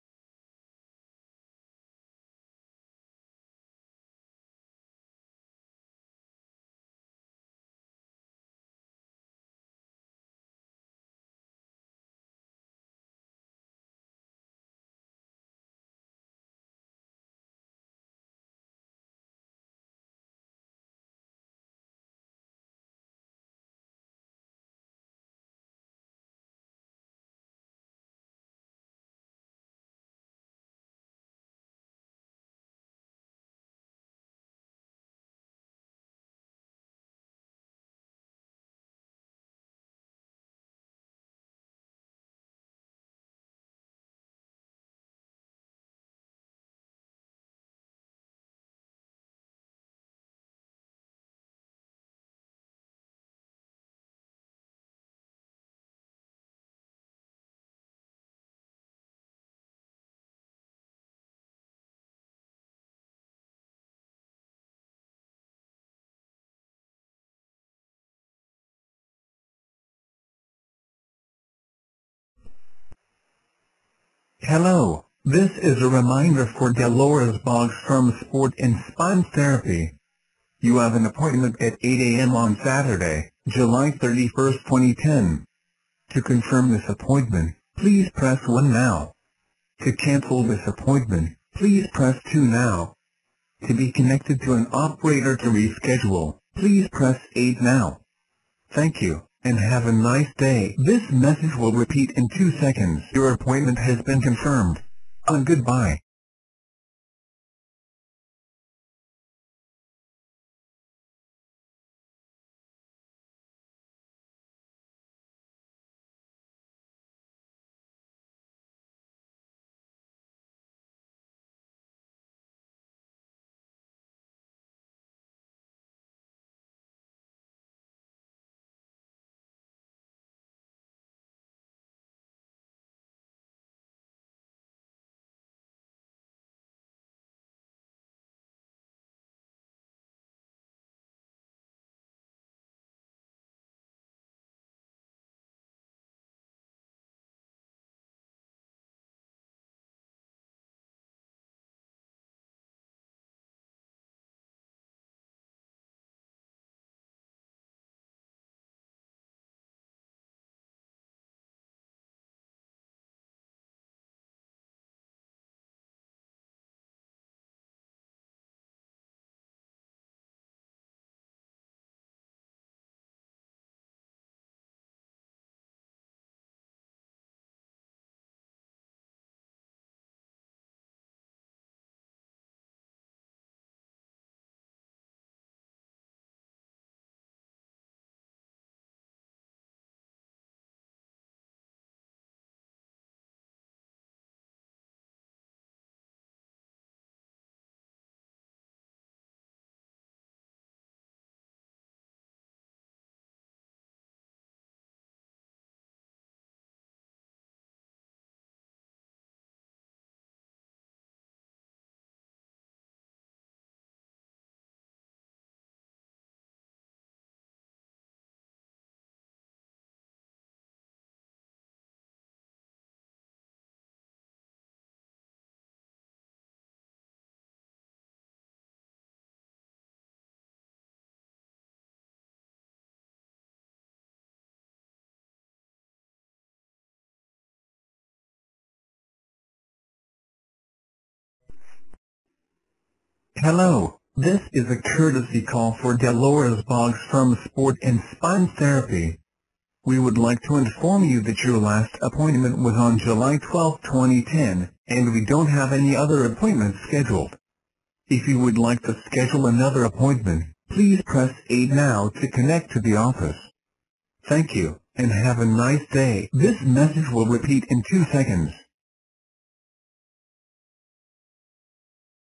TheraOffice & ReminderCall.NET Integration
This video demonstrates TheraOffice and ReminderCall.NET integration for sending patient reminder calls.